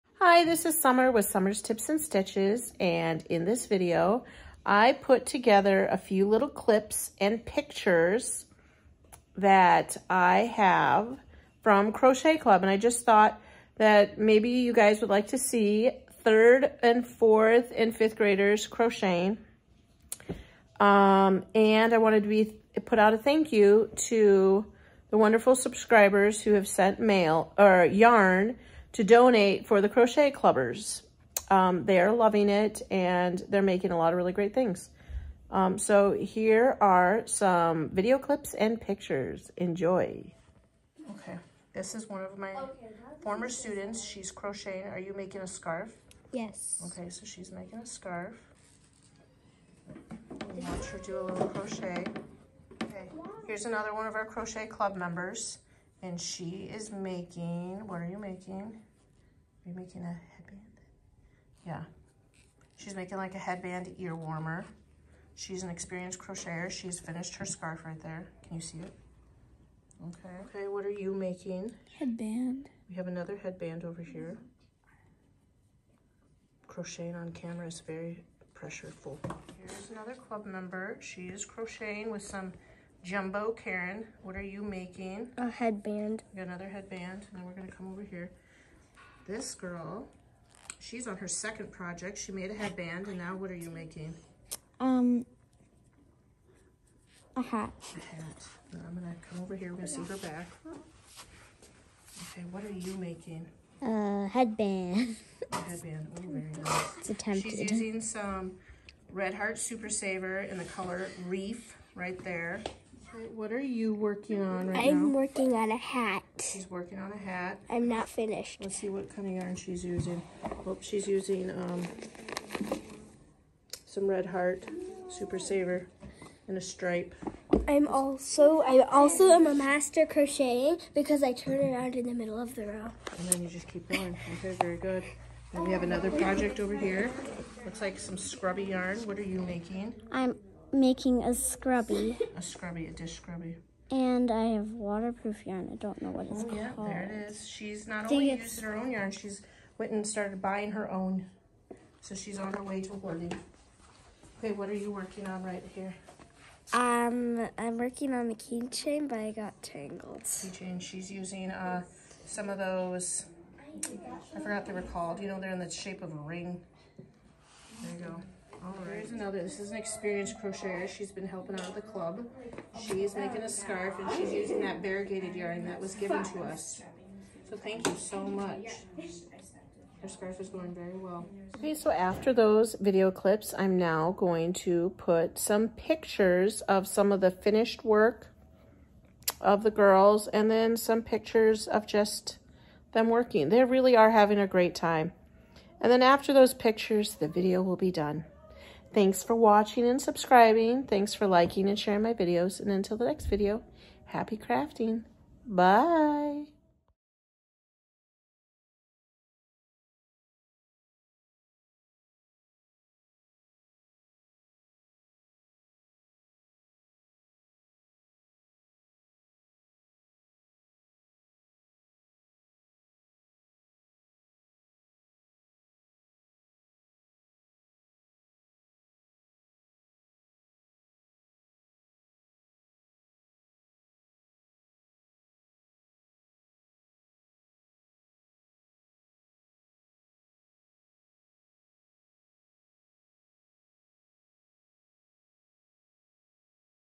Crochet Club
In this video I share a few clips and pictures from my crochet club at school. There are a few girls in this club that were in crochet club last year and I have a few first timers. We are working on making hats, scarves, dish scrubby‘s. I hope you enjoy their work. Thank you so much to everyone who has donated yarn to these girls.